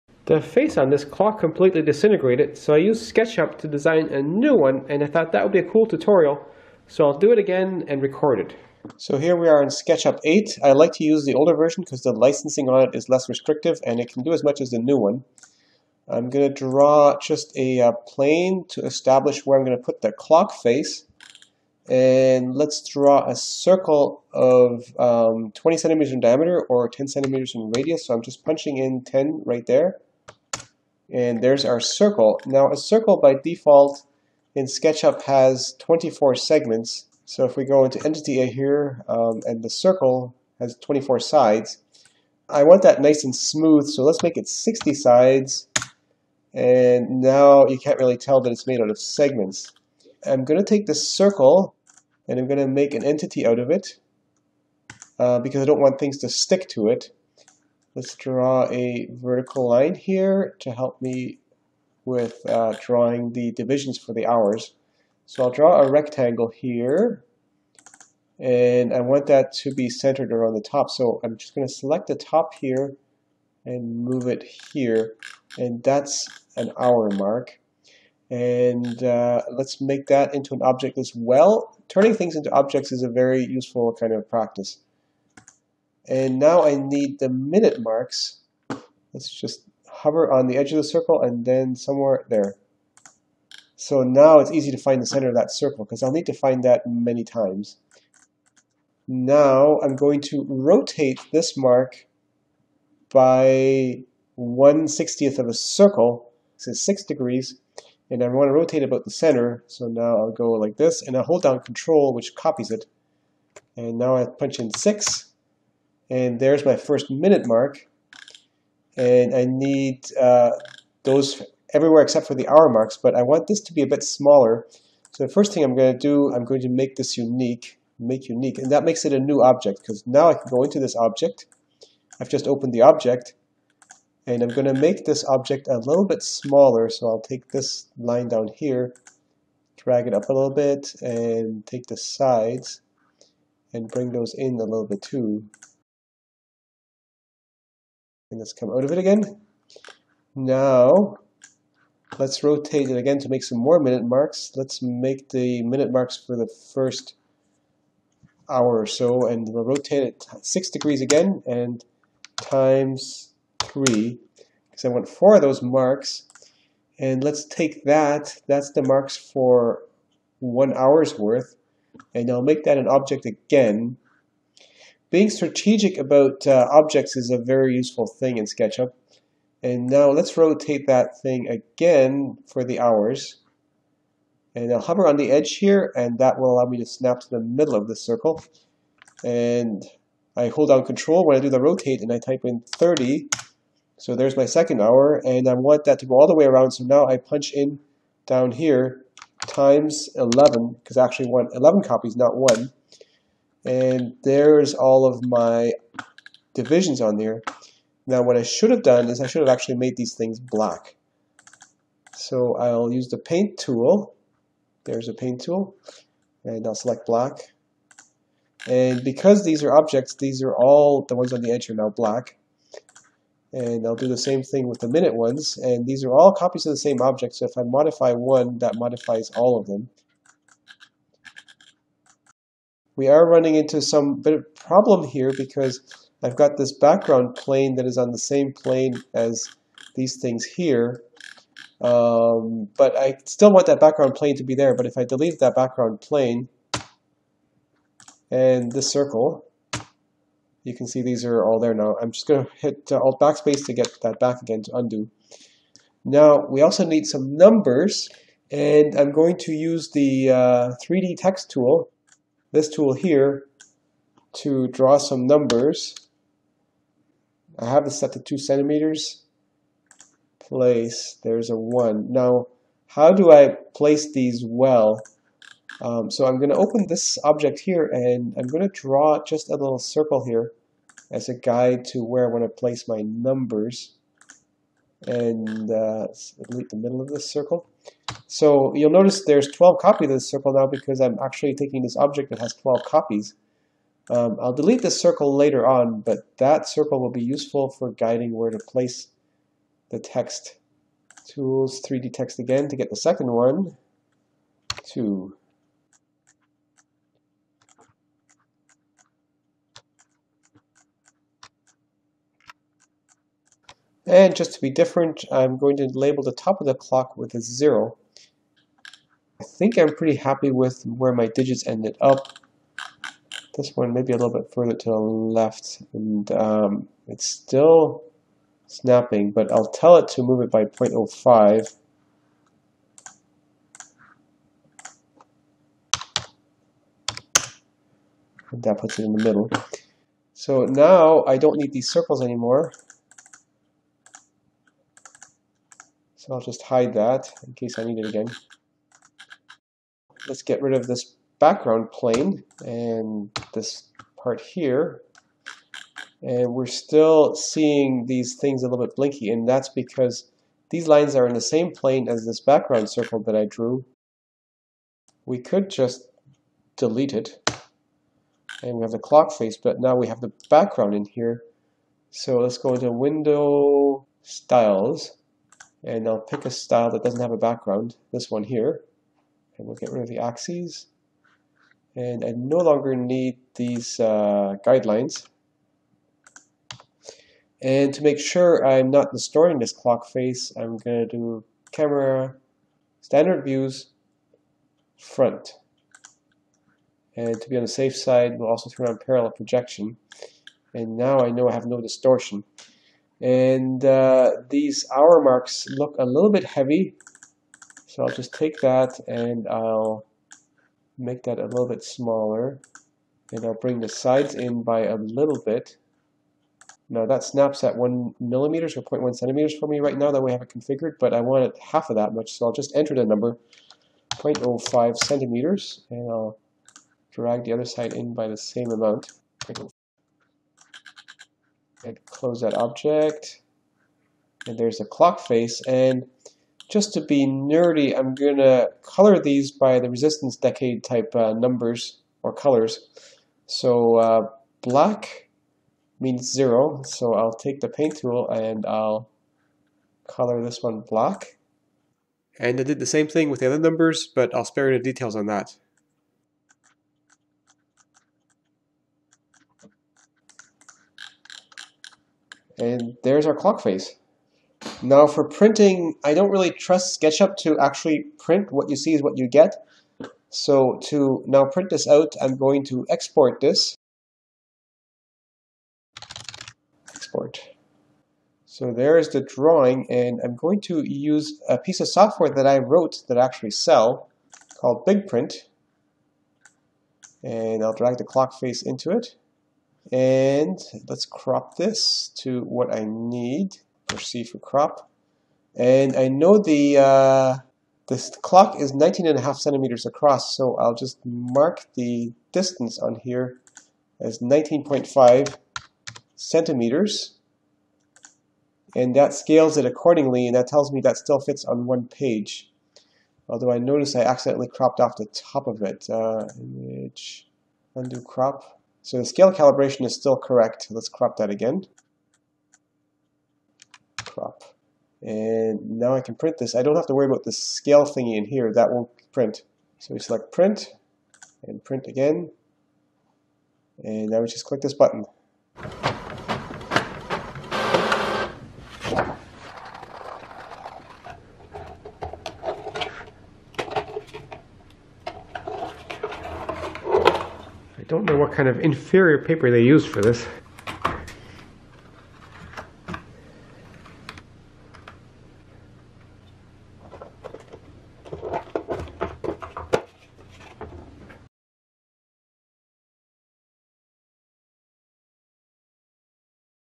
SketchUp: Drawing a clock face (repair for IKEA clock)
Focus on circles, rotations, and using objects.
Making a new clock face for my IKEA clock with a disintegrated clock face.